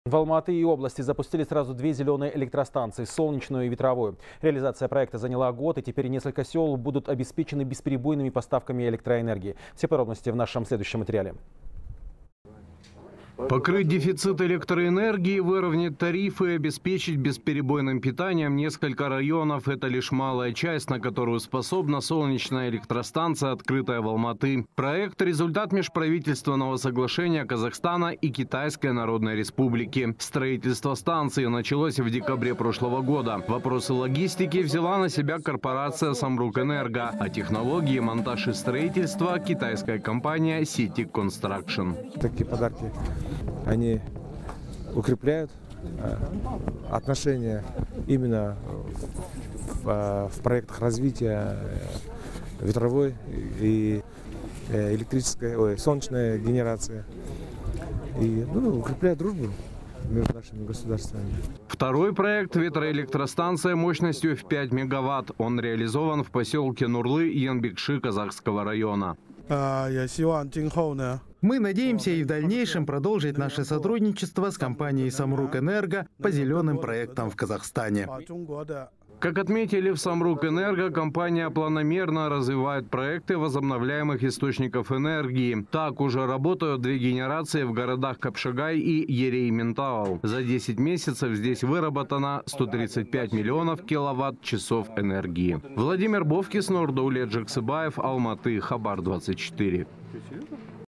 Две «зелёные» электростанции запустили в Алматинской области
В Алматы и области запустили сразу две «зелёные» электростанции: солнечную и ветровую, сообщает корреспондент «Хабар 24»